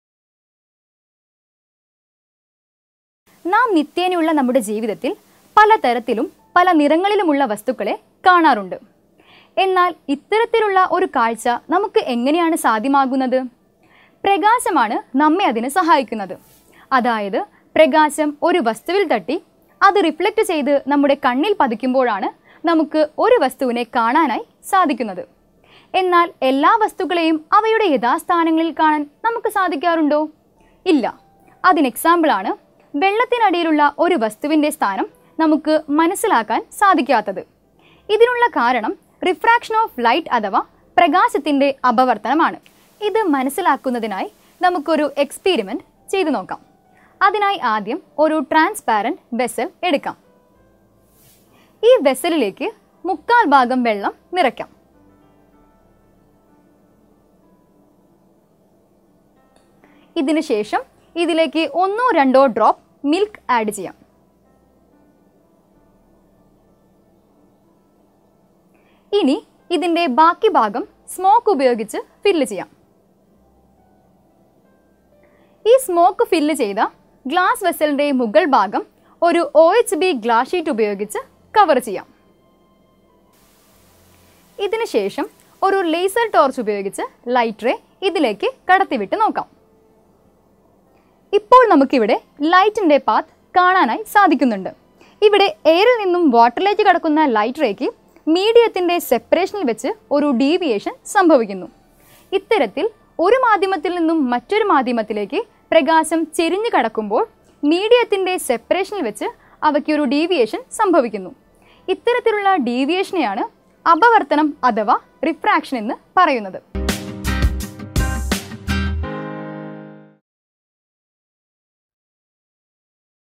Kerala Syllabus | Refraction of Light - 10th Standard - Physics - Kerala State Syllabus | 90+
Kerala State Syllabus (English, Malayalam Medium) / 90 Plus My Tuition App
The content and animations used in the above video is strictly based on the Kerala State Syllabus 10th standard Physics textbook. There may be different explanations on the particular topic in higher grades. But this video is purely concentrating on the 10th standard Physics syllabus.